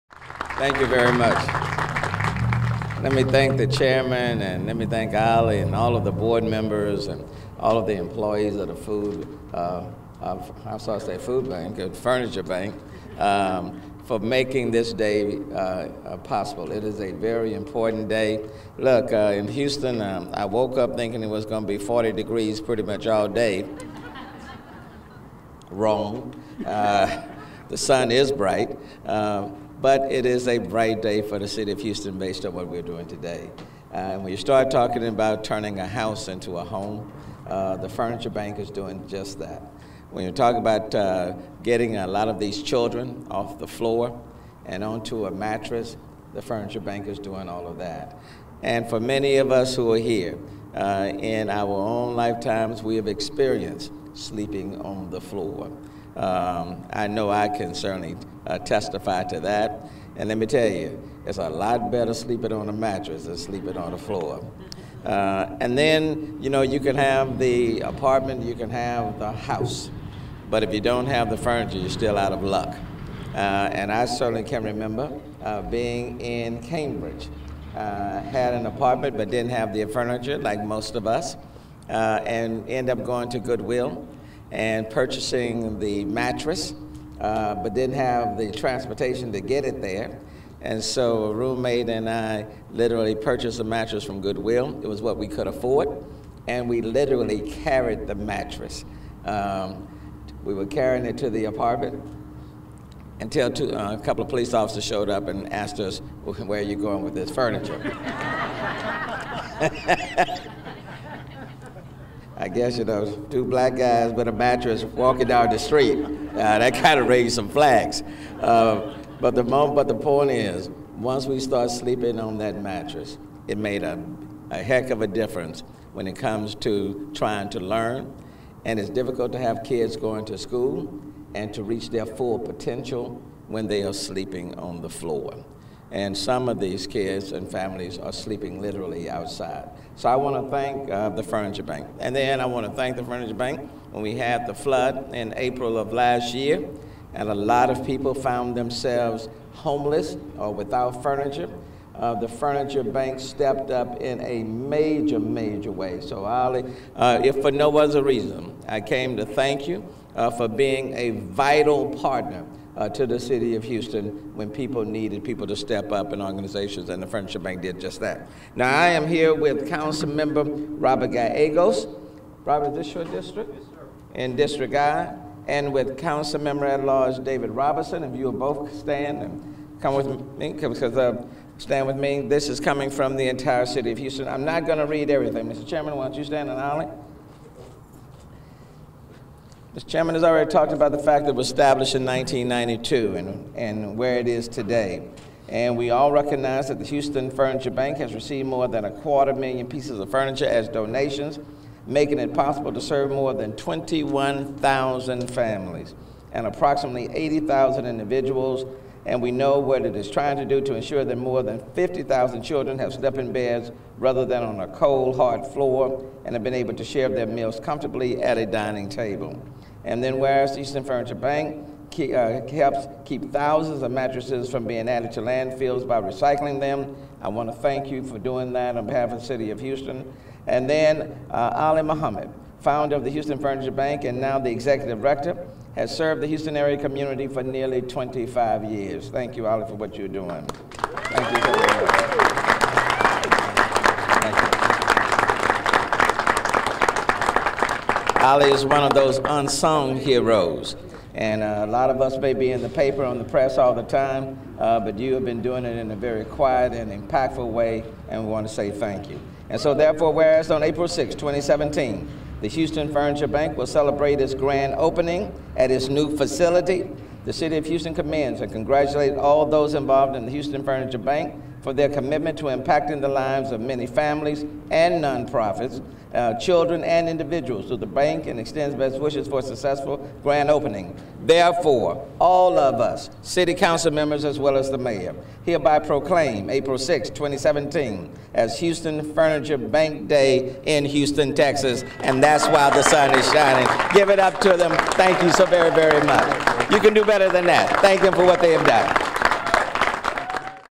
Mayor Turner Proclaims April 6, 2017 as Houston Furniture Bank Day
At a special grand opening ribbon cutting ceremony, Mayor Sylvester Turner offered kind remarks and a proclamation to Oli Mohammed, executive director of the Houston Furniture Bank.

The grand opening comes as the result of a two-alarm fire that destroyed the Houston Furniture Bank warehouse facility at 2100 Hussion Street back in August 2015.